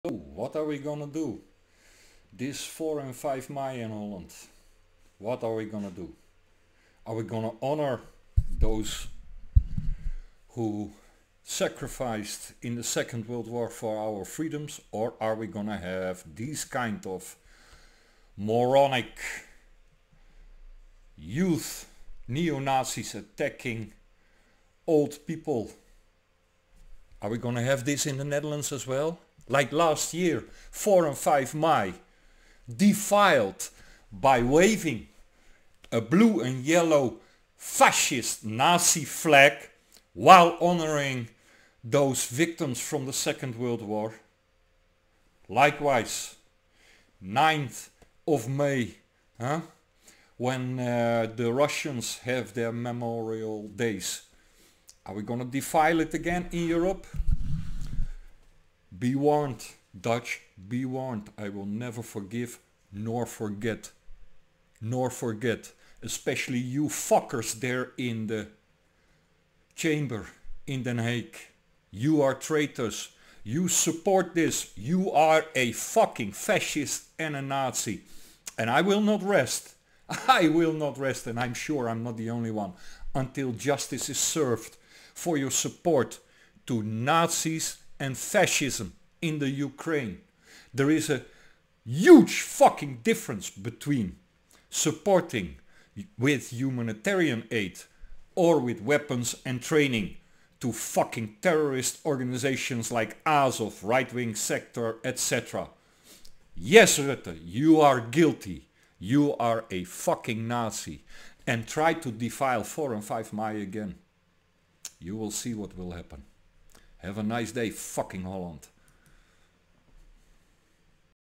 Are we going to allow this also in the Netherlands
The Netherlands has shown last year, 4 and 5 mei that they do not give a shit about the sacrifices made in the WW2 for our freedoms in the support they give to the Kiev Regime.
Rutte and its regime have shown what they are; Nazis